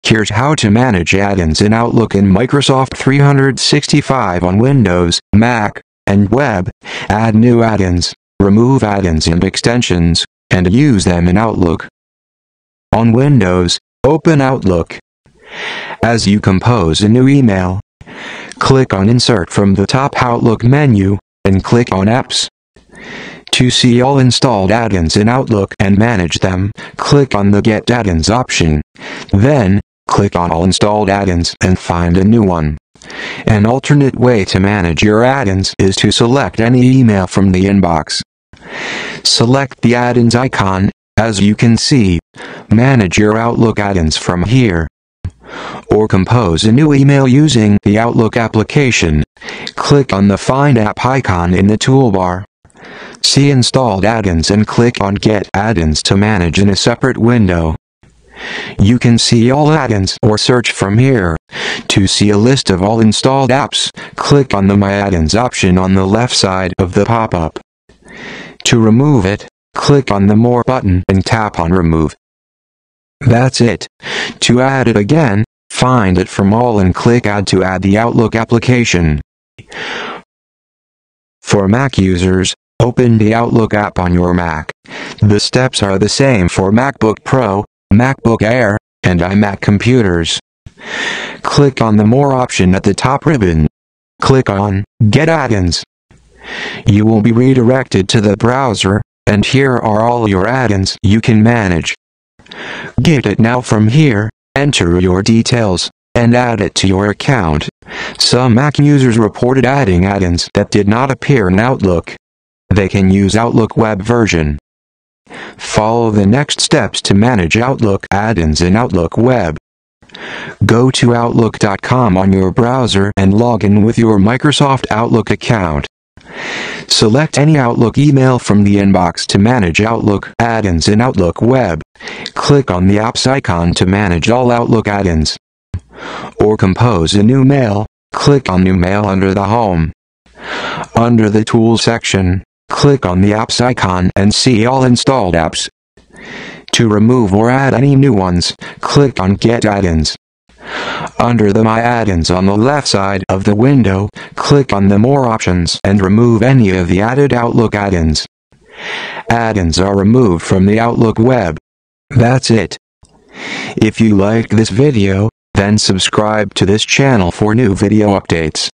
Uninstall Outlook Add-ins Windows, Mac, Web | Remove Add-ins from Outlook Microsoft 365
Here's how to Uninstall Outlook Add-ins on Windows, Mac, and Web  | Remove Add-ins from Outlook Microsoft 365. Force remove, add or Access Extensions in Outlook Windows, MacOS, and Microsoft.com - web version.

00:00 intro
00:14 Windows: Outlook Add-ins
01:23 Mac: Outlook Add-ins
01:58 Web: Outlook Add-ins

manage Add-ins in Outlook in Microsoft 365 on Windows, Mac, and Web: Add new add-ins, remove add-ins and Extensions, and use them in Outlook.

On Windows, open Outlook.

As you compose a New email,

Click on Insert from the top Outlook menu, and click on apps.

To see all installed Add-ins in Outlook and manage them, click on the Get Add-ins option. Then, click on All Installed Add-ins and find a new one.

An alternate way to manage your add-ins is to select any email from the inbox.

Select the add-in's icon, as you can see. Manage your Outlook add-ins from here.

Or compose a new email using the Outlook application. Click on the find app icon in the toolbar,

See Installed add-ins and click on Get add-ins to manage in a separate window.

You can see all add-ins or search from here. To see a list of all installed apps, click on the My Add-ins option on the left side of the pop-up.

To remove it, click on the more button and tap on remove.

That's it. To add it again, find it from All and click Add to add the Outlook application.

For Mac users, open the Outlook app on your Mac. The steps are the same for MacBook Pro, MacBook Air, and iMac computers.

Click on the More option at the top ribbon. Click on "Get Add-ins".

You will be redirected to the Browser, and here are all your add-ins you can manage.

Git it now from here, enter your Details, and add it to your account. Some Mac users reported adding add-ins that did not appear in Outlook. They can use Outlook Web version.

Follow the next steps to manage Outlook Add-ins in Outlook Web.

Go to Outlook.com on your Browser and log in with your Microsoft Outlook account.

Select any Outlook email from the inbox to manage Outlook add-ins in Outlook web. Click on the Apps icon to manage all Outlook add-ins.

Or compose a New mail, click on New mail under the Home.

Under the Tools section, click on the apps icon and see all installed apps.

To remove or add any new ones, click on Get add-ins.

Under the My add-ins on the left side of the window, click on the More Options and remove any of the added Outlook add-ins.

Add-ins are removed from the Outlook web. That's it.